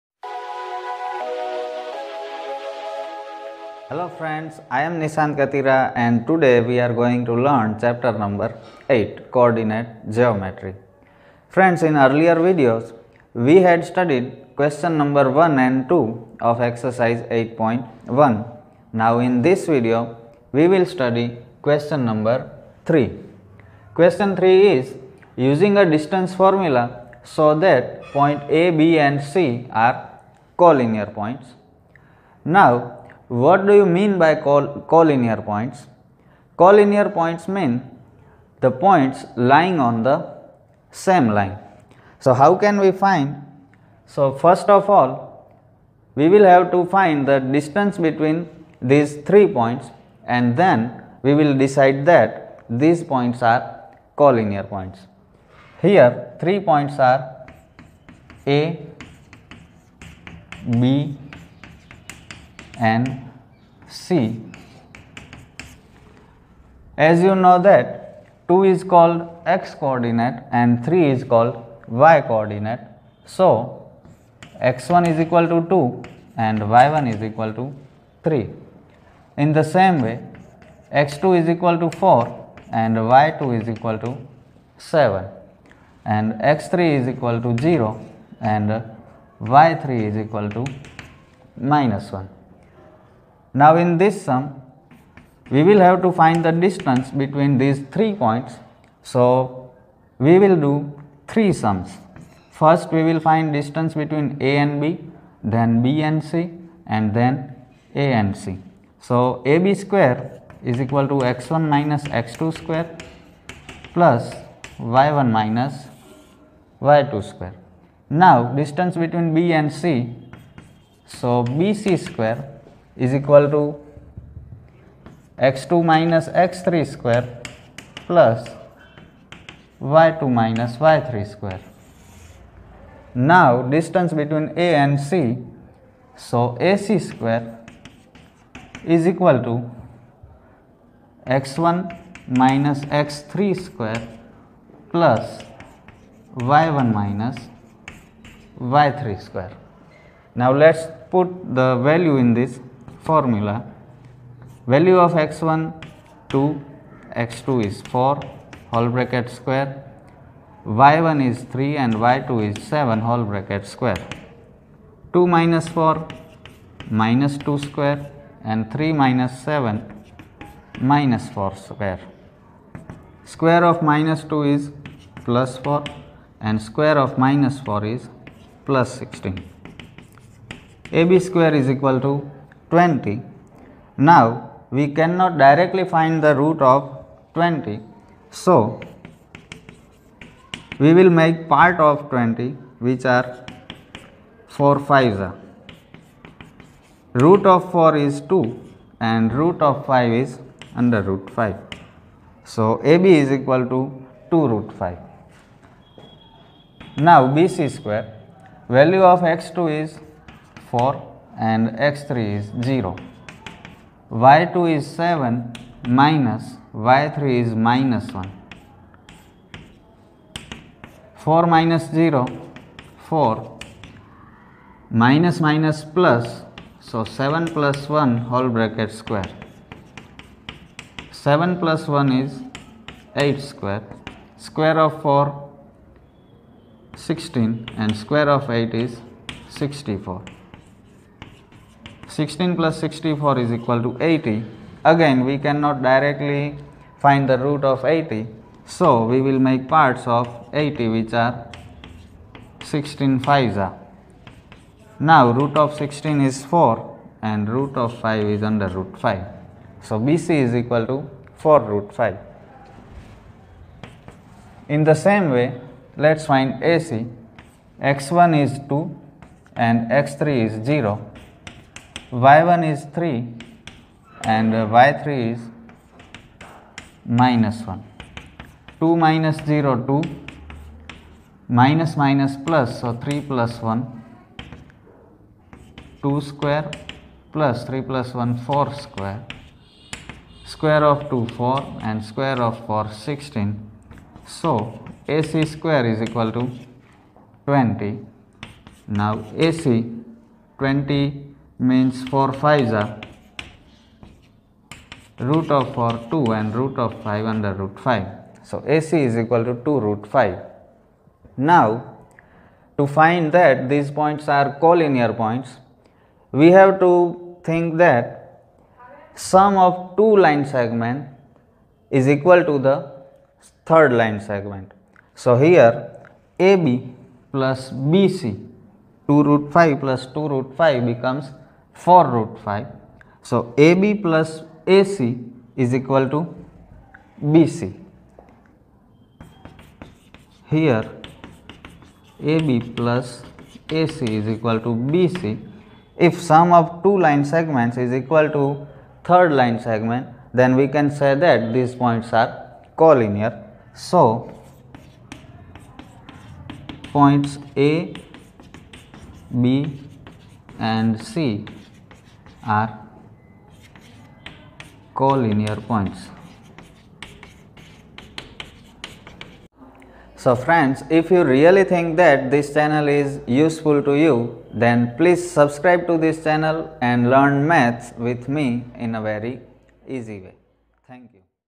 Std.10,Maths,Chapter-8 Coordinate Geometry,Exe-8.1,Q-3(GSEB)
Learn important sums of chapter 8 Coordinate Geometry in a very easy way.